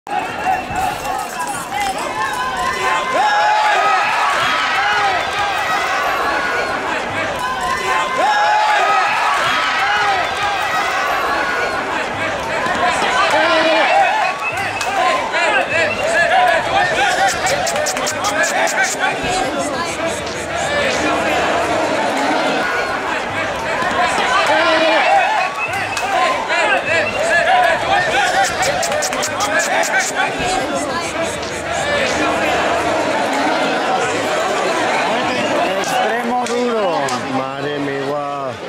✅2024 PICASSENT Toros y cerveza " NO "
2024 PICASSENT  SUSTO SIN CONSECUENCIAS,  MALA COMBINACION Toros y cerveza " NO "

                                   🟢  Reportaje SIN Maltrato animal ❤️✅